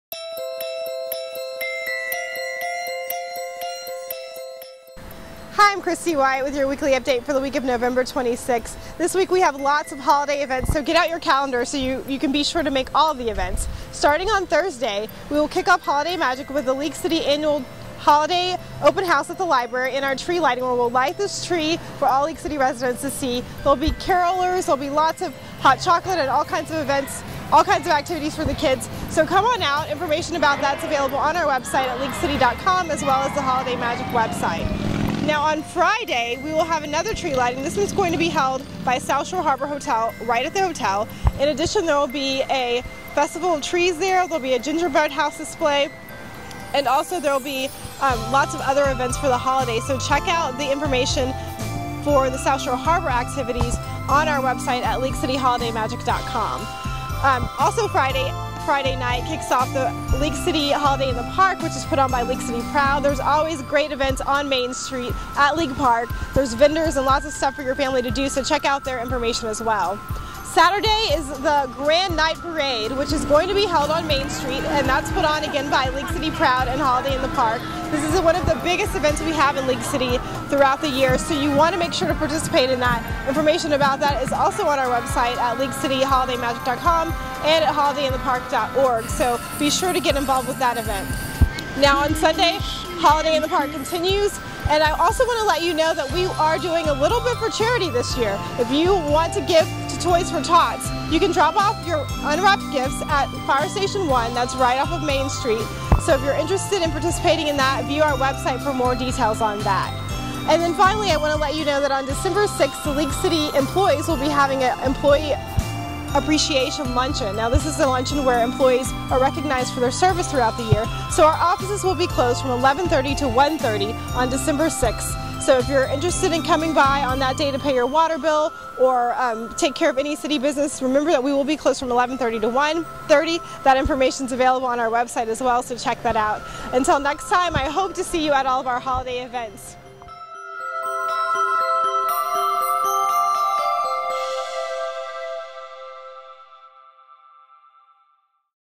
Weekly Upate November 26, 2012
The holidays are upon us and Holiday Magic kicks off this week with our tree lighting at Helen Hall Library! There's a packed weekend of events, including the grand night parade on Saturday, so be sure to tune in and get caught up on all that's going on in League City this week! Happy Holidays!